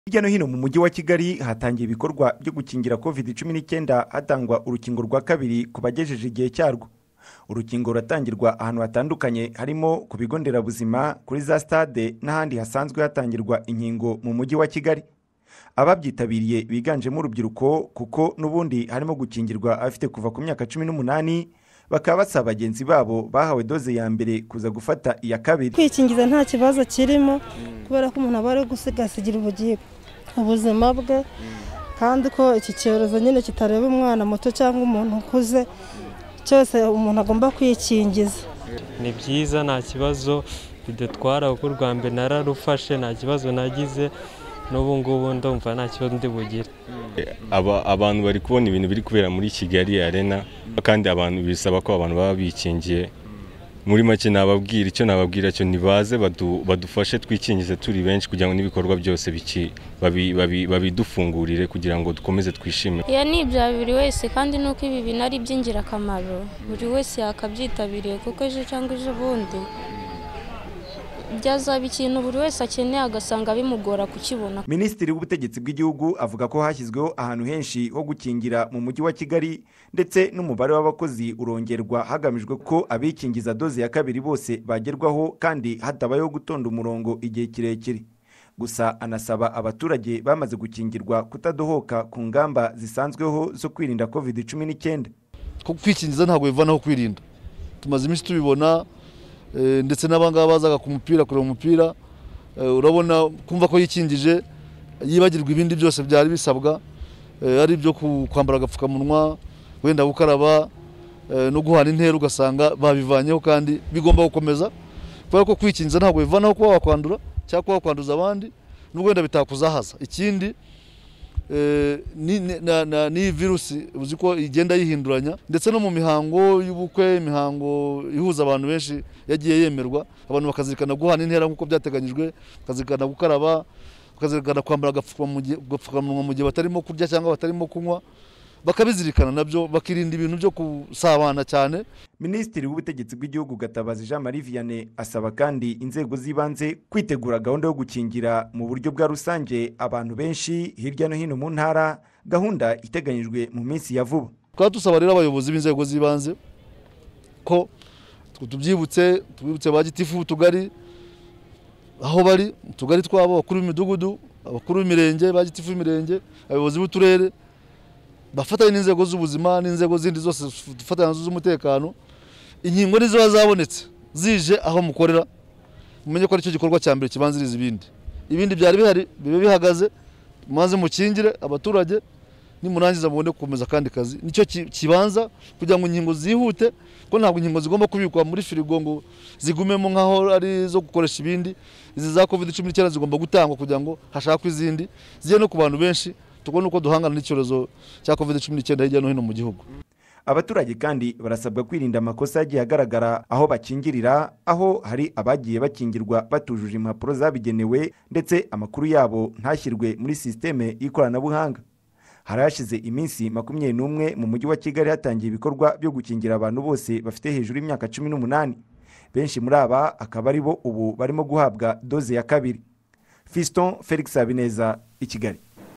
Ministiri Gatabazi yasabye abarebwa no gufata dose ya 2 y’urukingo rwa COVID-19 kwihutira kurufata
Ministiri w'ubutegetsi bw'igihugu Gatabazi JMV yasabye abanyarwanda barebwa no gufata dose ya kabiri y’urukingo rwa covid-19 kwihutira kurufata, kuko guhera kuri uyu wa kabiri mu mujyi wa Kigali ibikorwa byo gukingira byakomeje.